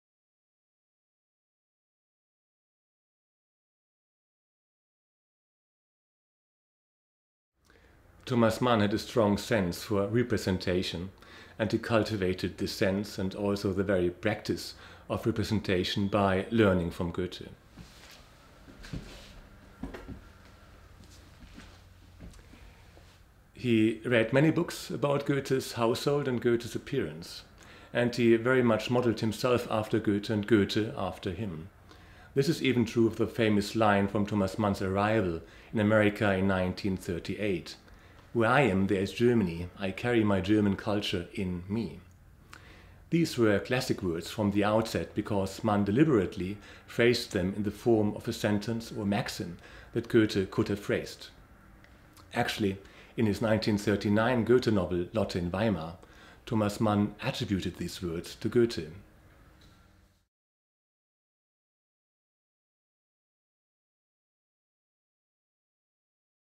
Stefan Keppler-Tasaki on the Role of Goethe for Thomas Mann
Thomas Mann Fellow Stefan Keppler-Tasaki talks about the role of Johann Wolfgang von Goethe for Thomas Mann at the Thomas Mann House, Los Angeles. The Thomas Mann House is an artists' residence and space for transatlantic debate.